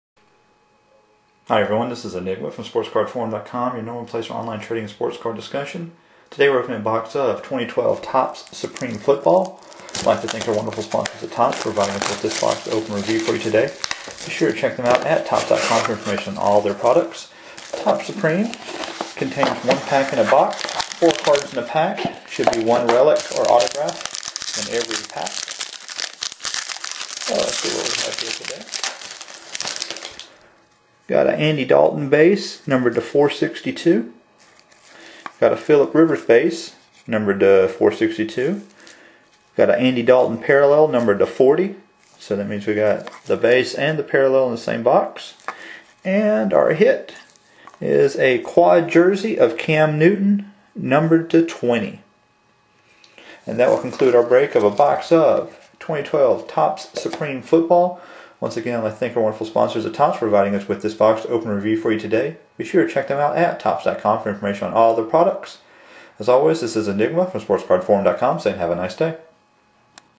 SportsCardForum.com Live Break: 2012 Topps Supreme Football Hobby Box
This is a live break of a box of 2012 Topps Supreme Football.  This box was provided by Topps to Sports Card Forum for reviewing purposes.  For more information on all of their products, visit them today